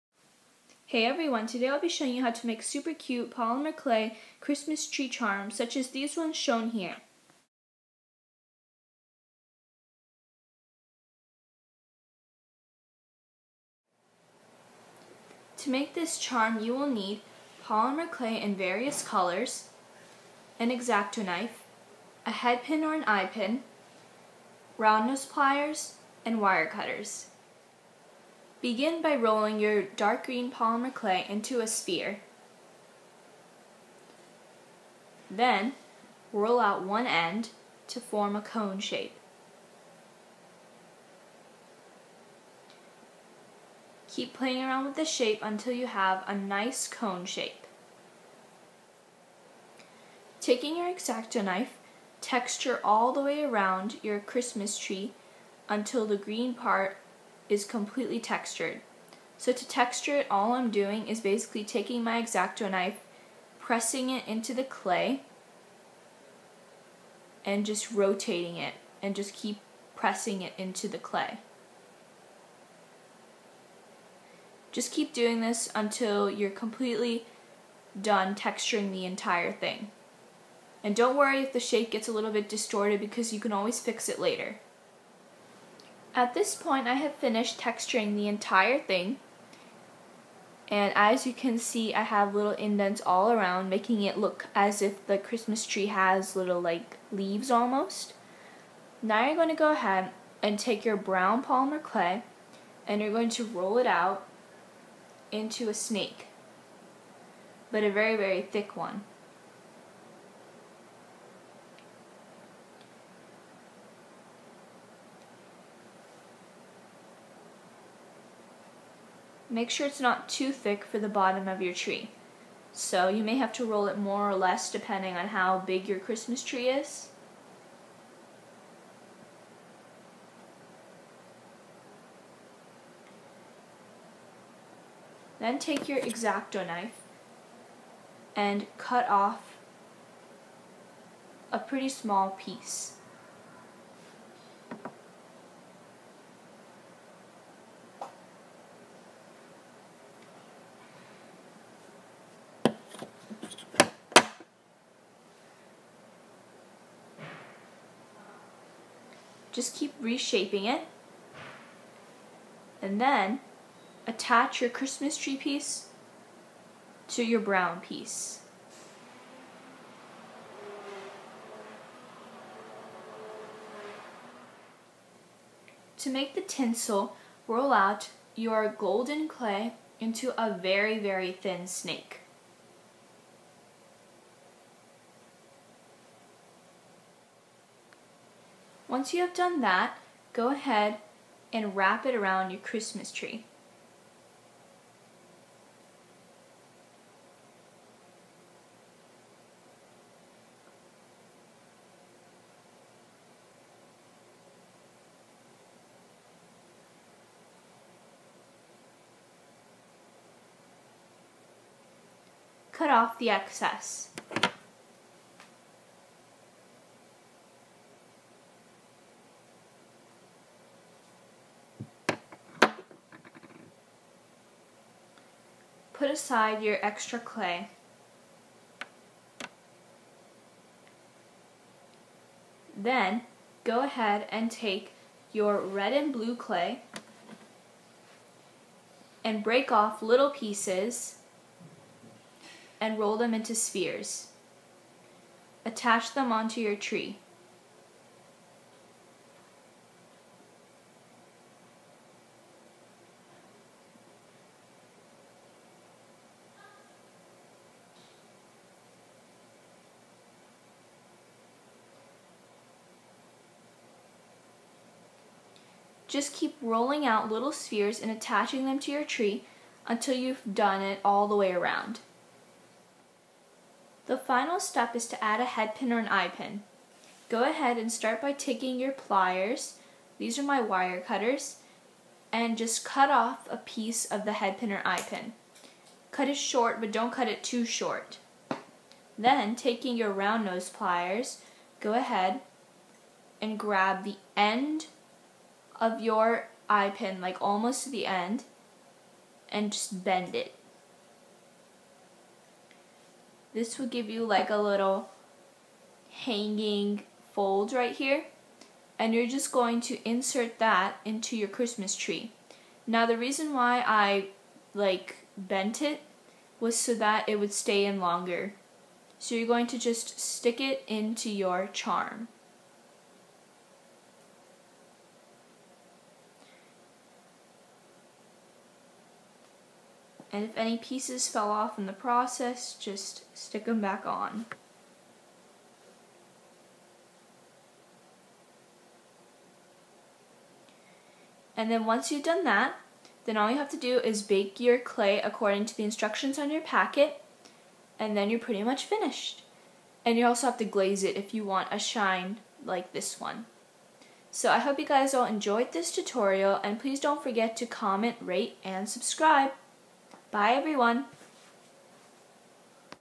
Christmas Tutorial: Polymer Clay Christmas Tree Charm
Enjoy! These charms are perfect to attach to a cellphone strap to give as gifts!